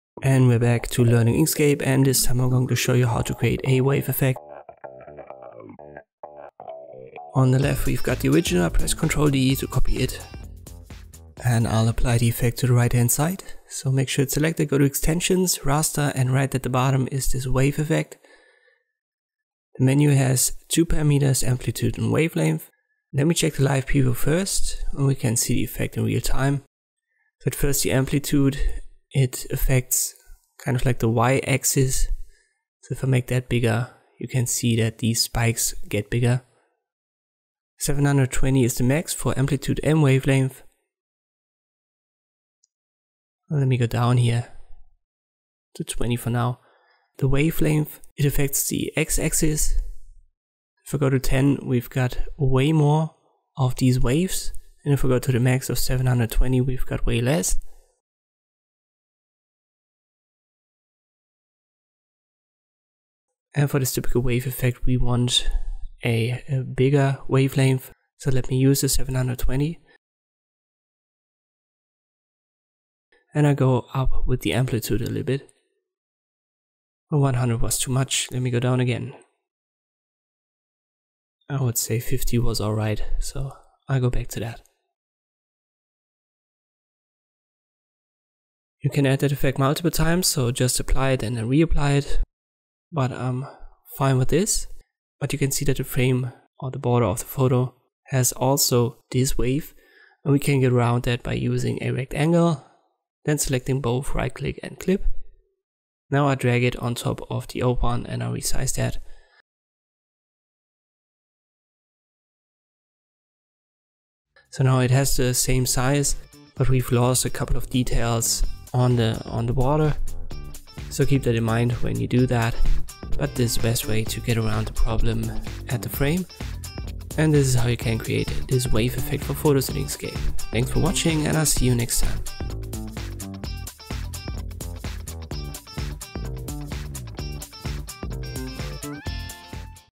How to create a wave effect for photos in Inkscape
In this tutorial I'll show you how to create a wave effect for photos in Inkscape.